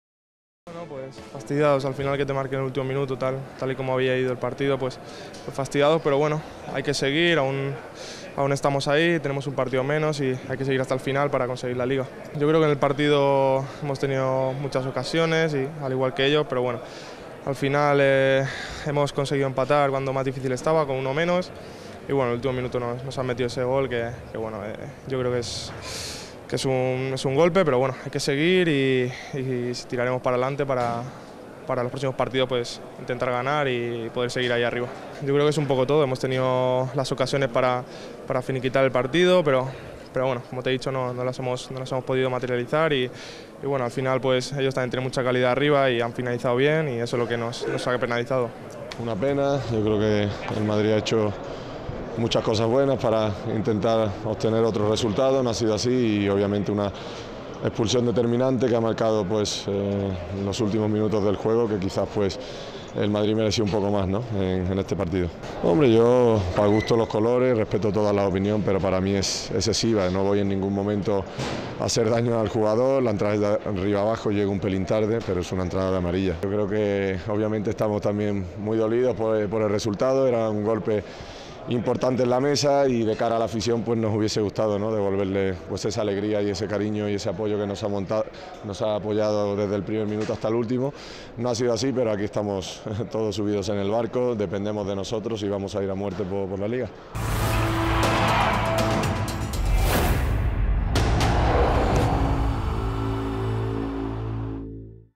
Sergio Ramos y Asensio valoraron la actuación del equipo en el Clásico
Ramos: “Vamos a ir a muerte a por la Liga”
Asensio: “Hemos tenido ocasiones para finiquitar el partido”